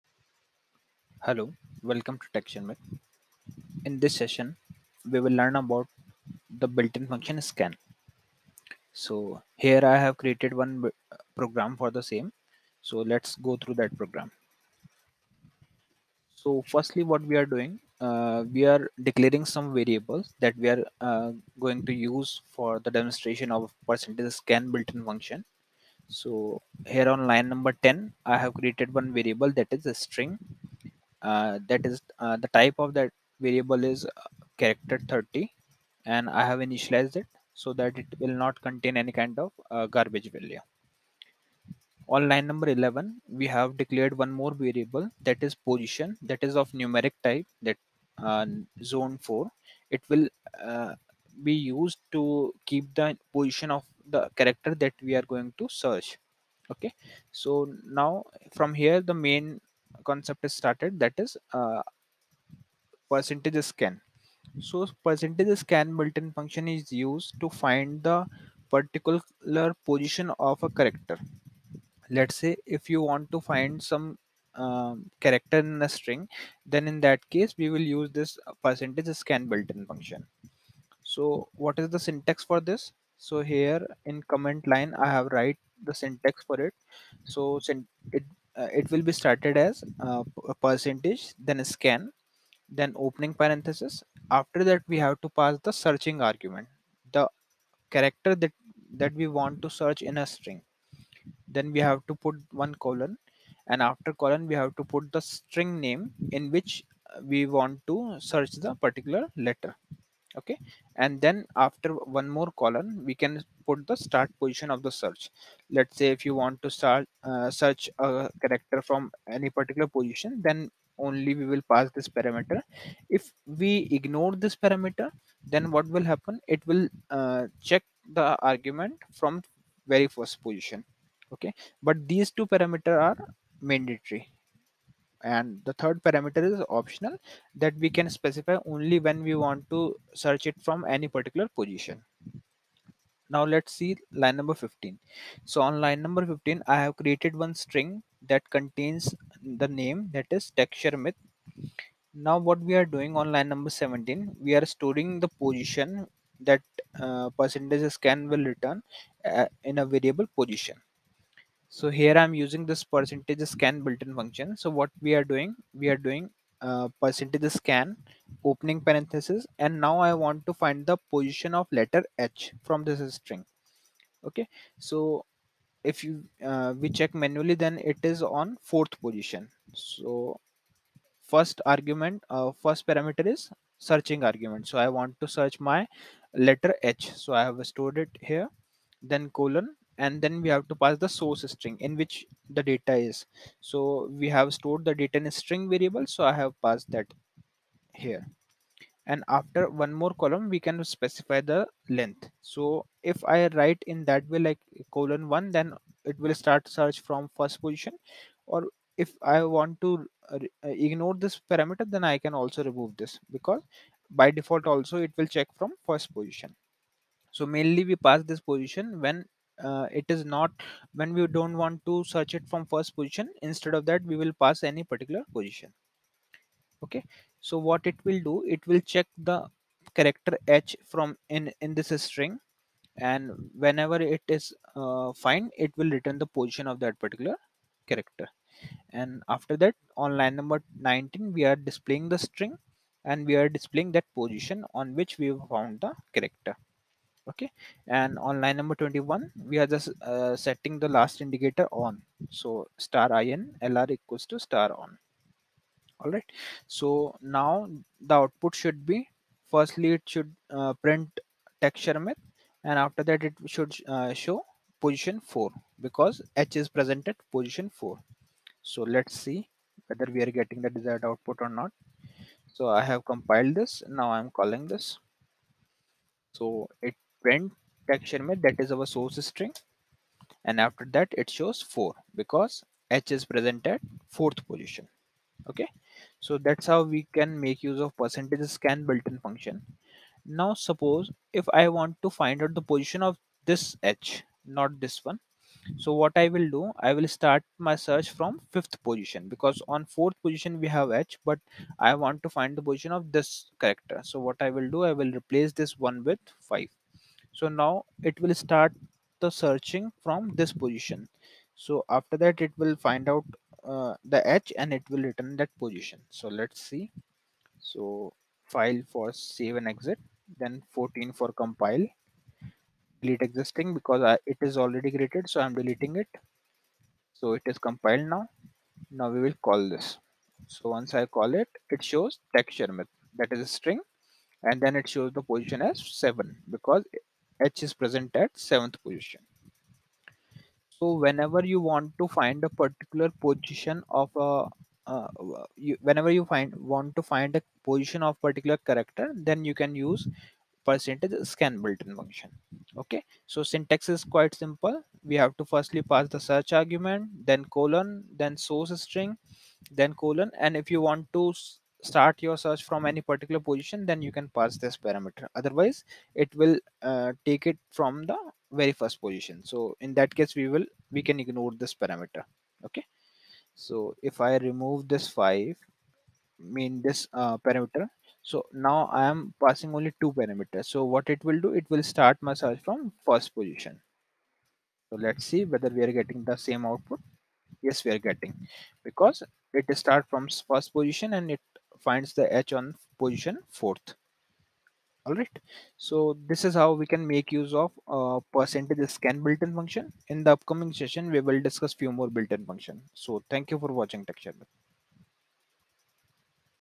IBMi (AS400) - %Scan Built in function in rpgle | rpgle programming tutorial
1. Introduction of %scan built-in function in RPGLE.
2. Example of %scan builtin function in RPGLE in English